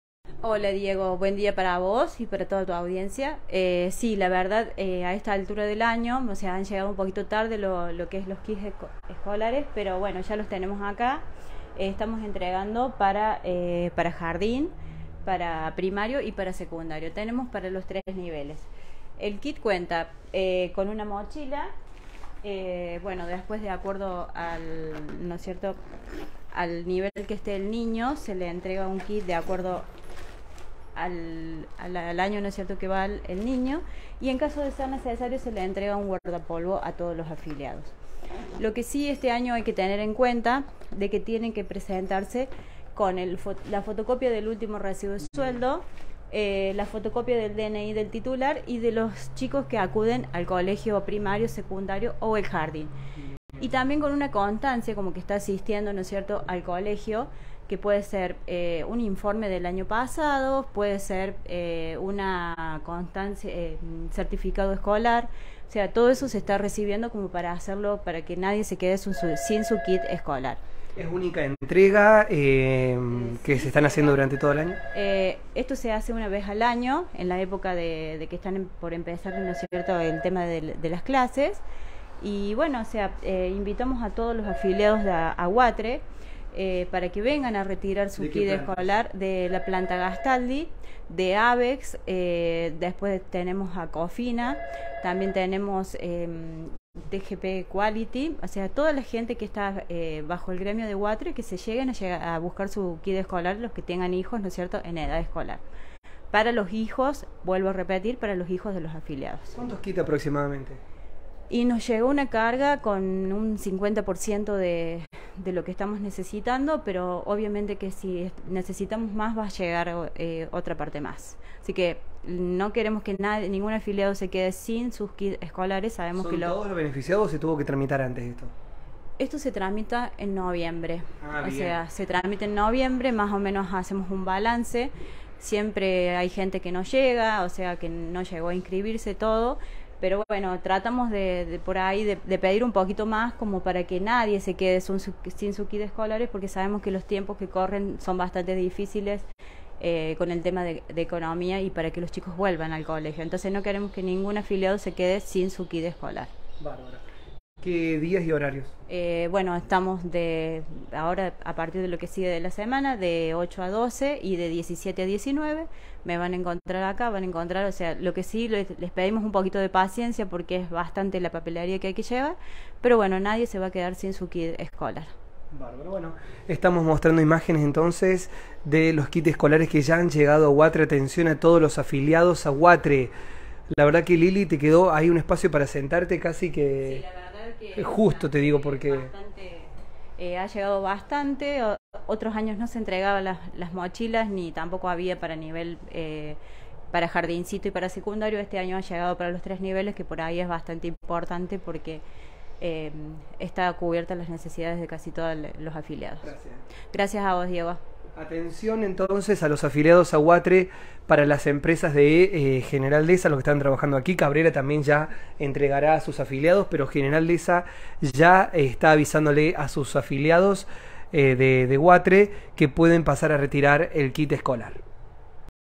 GENERAL DEHEZA: ENTREGA DE KIT ESCOLARES A LOS AFILIADOS DE U.A.T.R.E
Liliana Prado desde el móvil de Radio Vos.